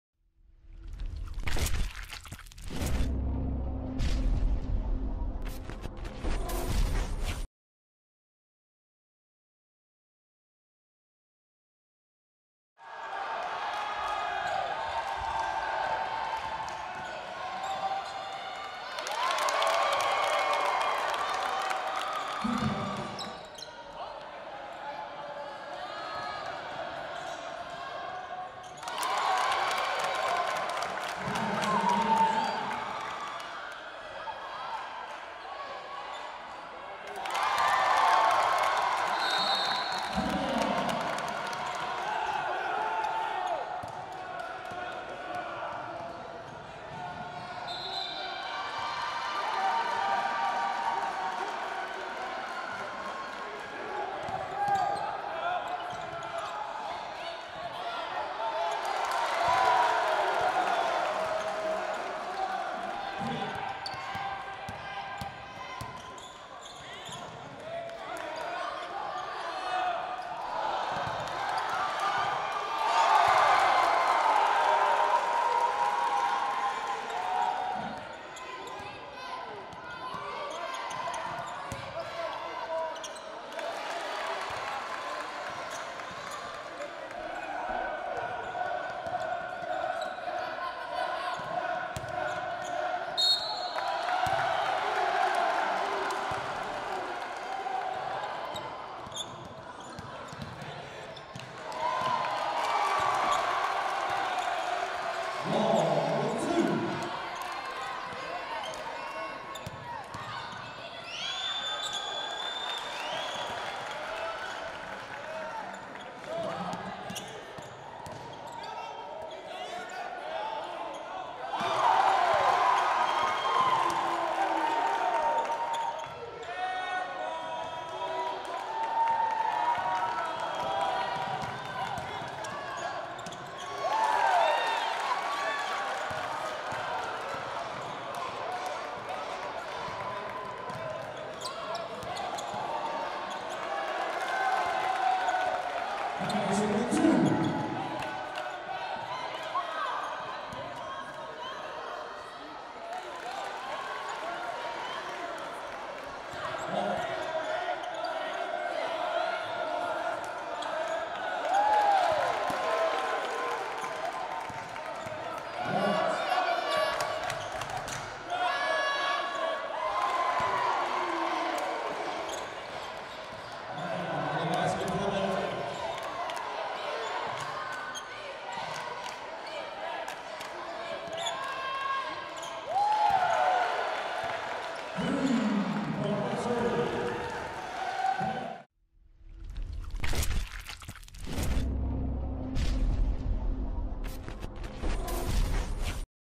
Jacksonville State Women's Basketball 2017-18 - vs. Brewton-Parker Highlights
Highlights from Jacksonville State's 103-44 win over Brewton-Parker on November 10 at Pete Mathews Coliseum.